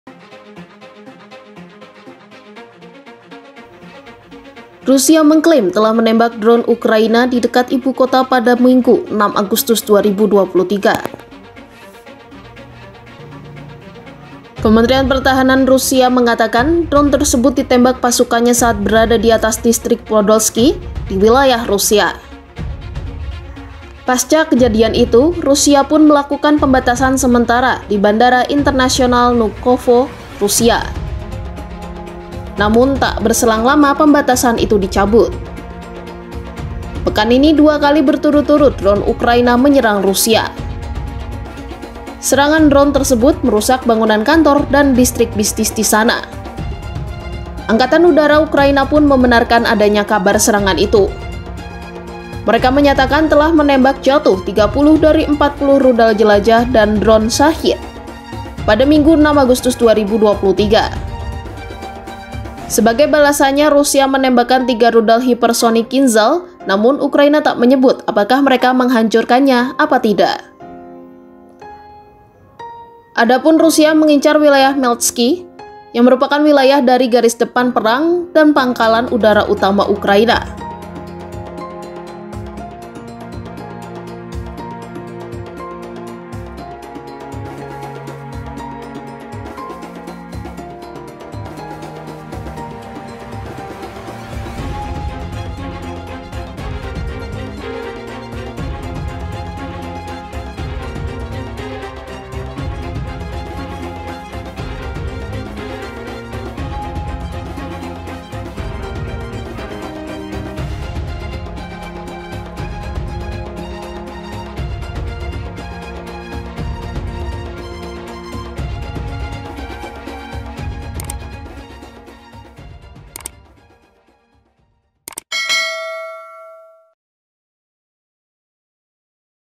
Rusia Klaim Tembak Jatuh Drone milik Ukraina
Wali Kota Moskwa Sergei Sobyanin pada Minggu (6/8/2023) mengatakan, pertahanan udara Rusia menghancurkan drone Ukraina di dekat ibu kota.
Moskwa jarang menjadi sasaran selama perang di Ukraina berlangsung, tetapi belakangan ini mendapat beberapa serangan.
"Hari ini sekitar pukul 11.00 sebuah drone berusaha menerobos masuk Moskwa. Itu dihancurkan oleh pasukan pertahanan udara saat mendekat," kata Sobyanin di Telegram, dikutip dari kantor berita AFP.
Penulis: Aditya Jaya Iswara
Penulis Naskah: Anneke Sherina Ramadhani
Narator: Anneke Sherina Ramadhani
Video Editor: Menika Ambar Sari
Produser: Ira Gita Natalia Sembiring